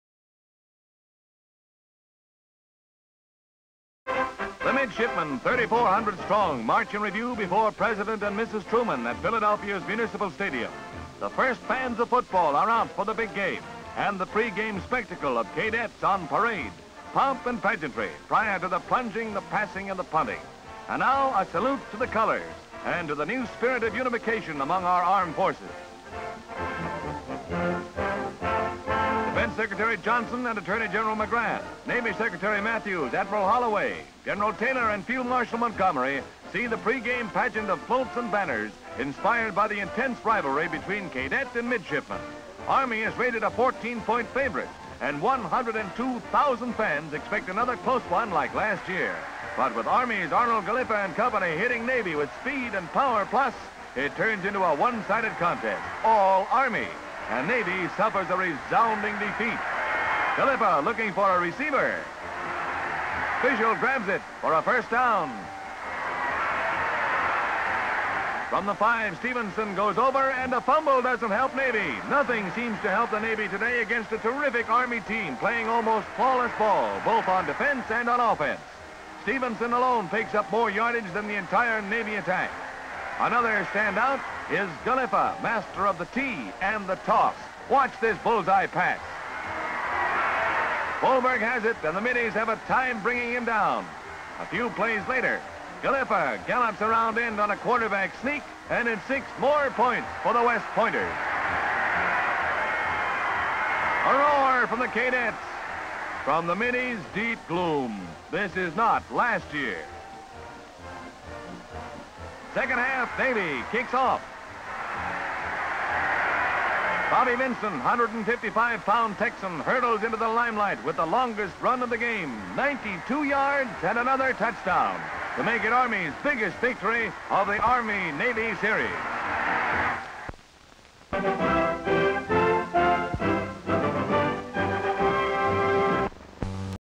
Dignitaries and spectators gathered to watch a football match between US Army and...HD Stock Footage
Dignitaries and spectators gathered to watch a football match between US Army and Navy in Philadelphia, Pennsylvania.

Large number of spectators gathered to watch a football match between teams of US Army and Navy in Philadelphia, Pennsylvania. The US Army and Navy soldiers parade before the start of the match. The US President Truman and other dignitaries watch among the spectators. Players show their tactics during the match. Players Galiffa and Stephenson run during the match. The Army defeats the Navy 38-0. Location: Philadelphia Pennsylvania. Date: November 28, 1949.

57,000+ broadcast-quality historic clips for immediate download.